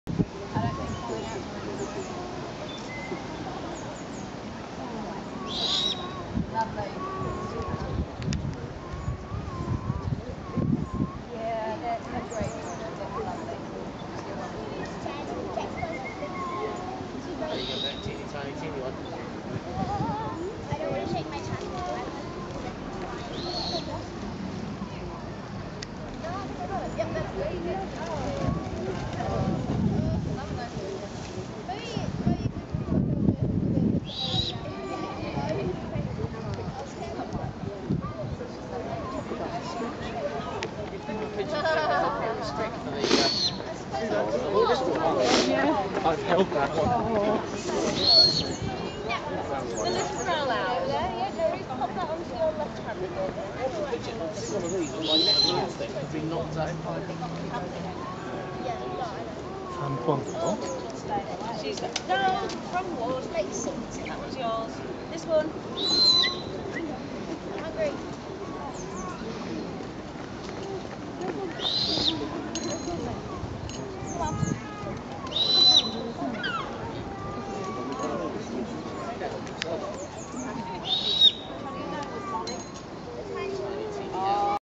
Owl Adventures YORK 1ST JULY 2015
Little sample of the lovely Owl Adventures in the gardens around york..